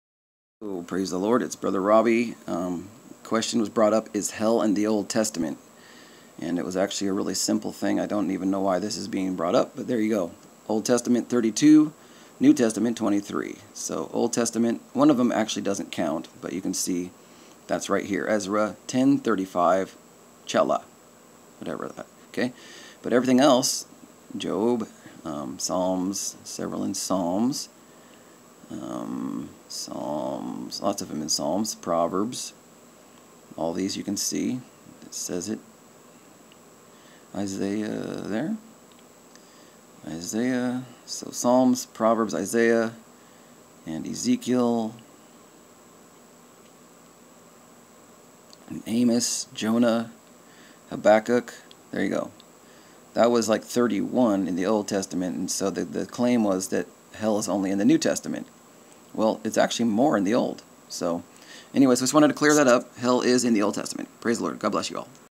Atheist attacks against Christians 1, Is Hell in the Old Testament?
yep, they try to play this card ..
.well here is the answer
31 times in the OLD TESTAMENT
23 times in the NEW
AMEN!